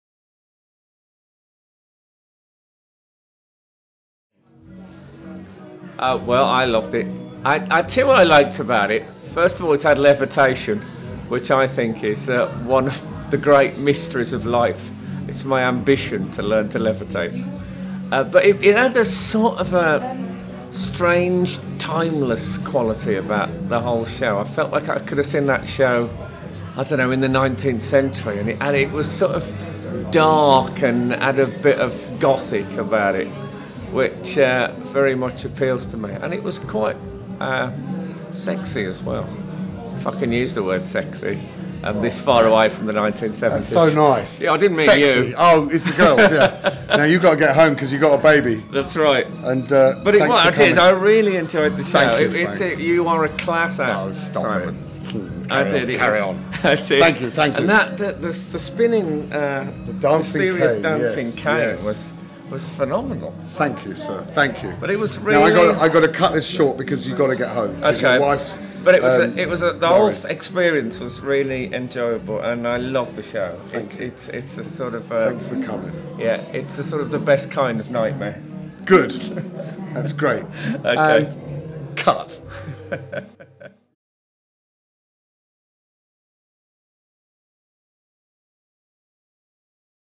Frank Skinner 13th December 2012 at Simon Drake's House of Magic
Frank Skinner comments right after the show on 13th December 2012
'The best kind of nightmare'
Thank you Frank!